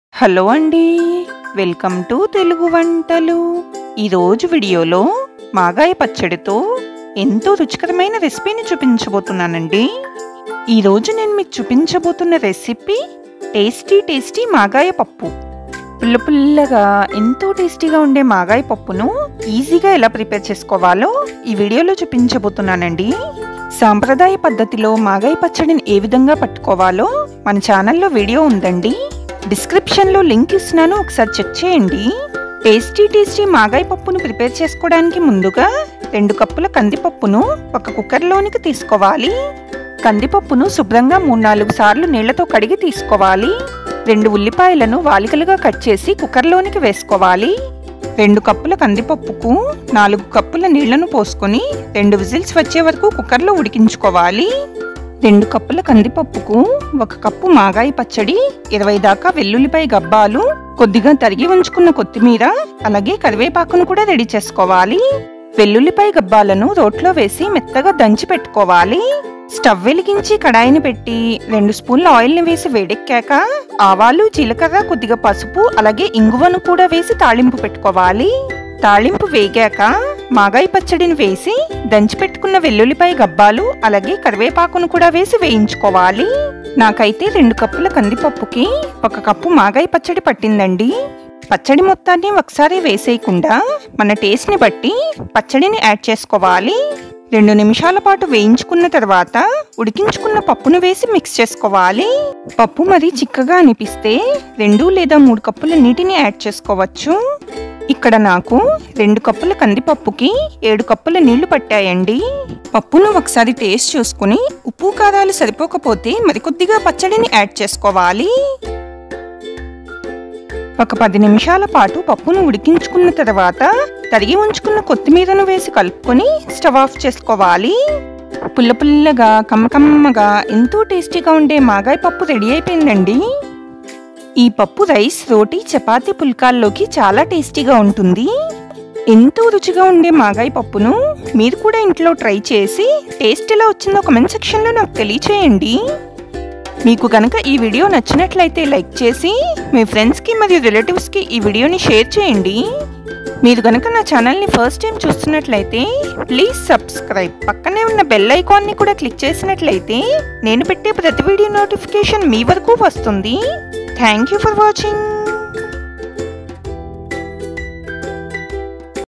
Mango pickle dal - Magaya pappu - మాగాయ పచ్చడితో ఇలా పప్పును చేసి చూడండి చాలా రుచిగా కమ్మగా ఉంటుంది
Magaya pappu